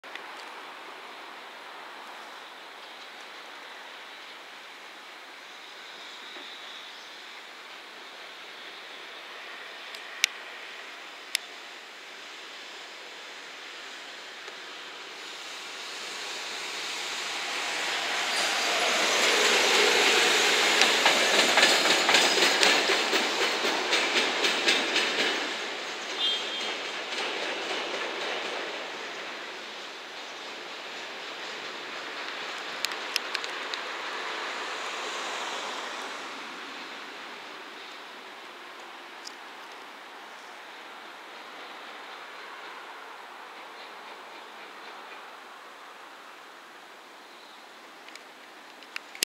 元気になった姿をご覧ください パンタ脱落から復活2日目 300番台トップ 山陽本線 上り 貨物列車 92レ EF210-301号機 単機 JR貨物 広島 2020.10.18 02196
パンタ故障から復活2日目 300番台トップ 山陽本線 上り 貨物列車 92レ  EF210-301号機 単機 JR貨物 広島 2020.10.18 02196

17日から営業再開